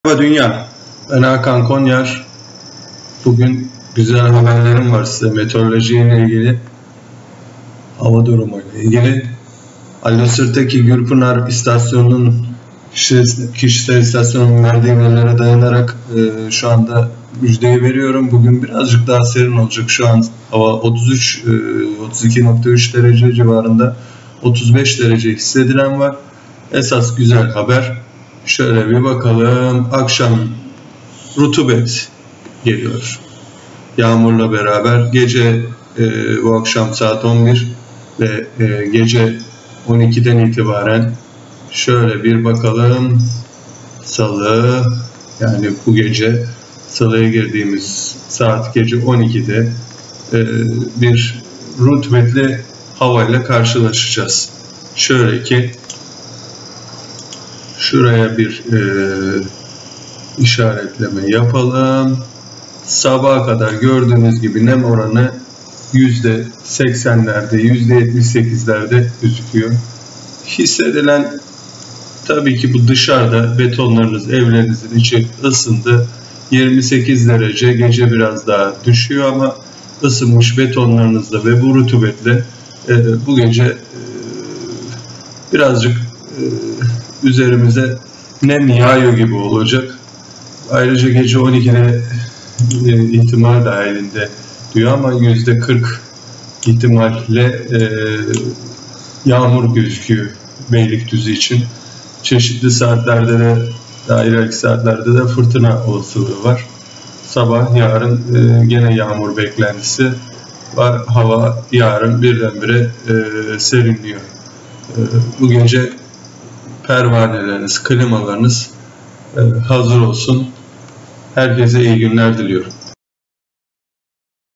03 04 07 2017 Nemli bir gece olacak Hava durumu Beylikdüzü İstanbul
Bu gece İstanbul nemli olacak .Elektrikler kesilmese bari